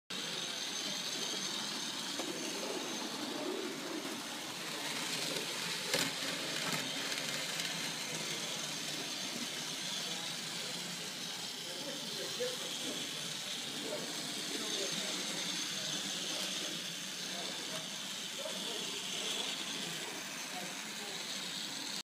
Evan Z's World's Fair N scale running
Evan's well appointed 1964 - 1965 World's Fair trainset takes a spin on the newly restored RMLI N scale layout.  Thanks to Francis for making this possible with all the restoration work over the winter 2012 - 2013.  This is a new addition to RMLI Riverhead,